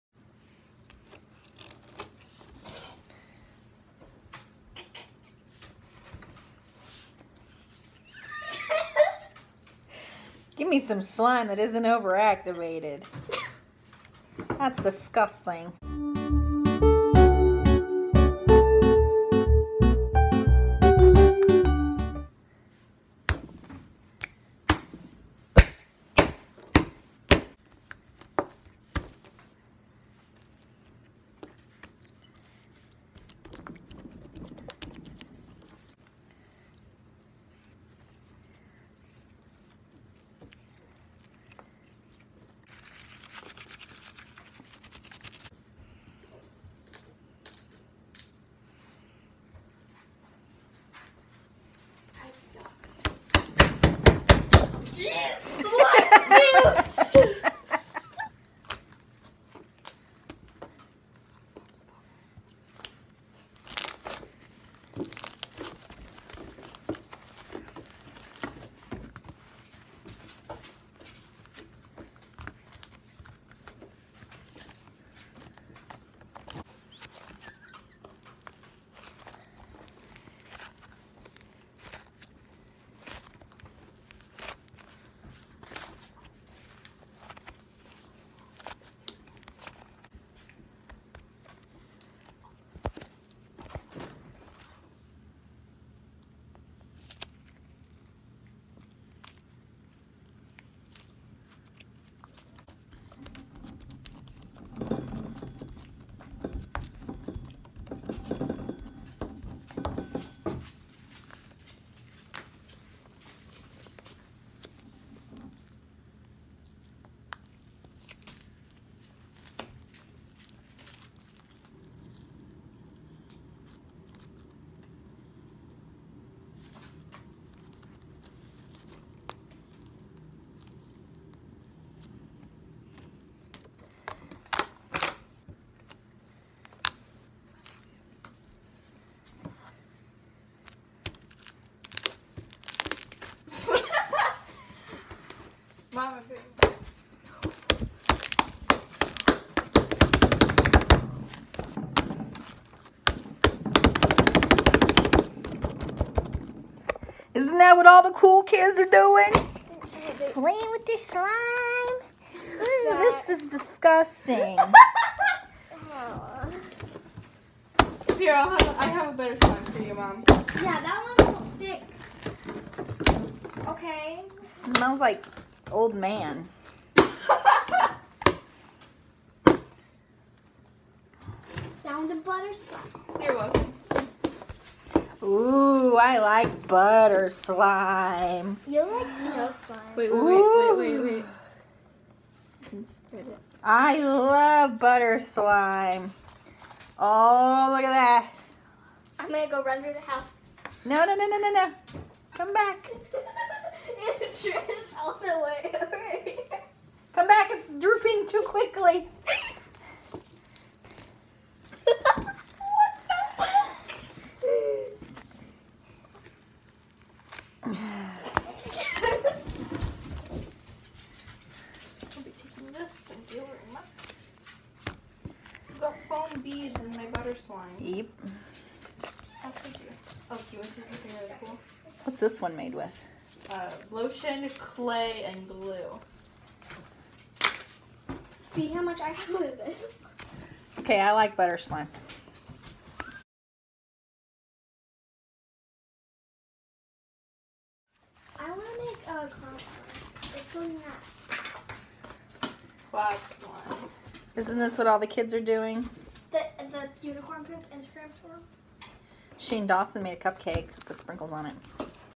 Spanking slime. Cuz it’s gross.
Slime is so gross.  My kids have made a ton of it and we spent some time playing with it.   Cuz that’s why cool kids do.  Blecccc.    For some reason I can’t stop smacking it.   🤷🏼‍♀️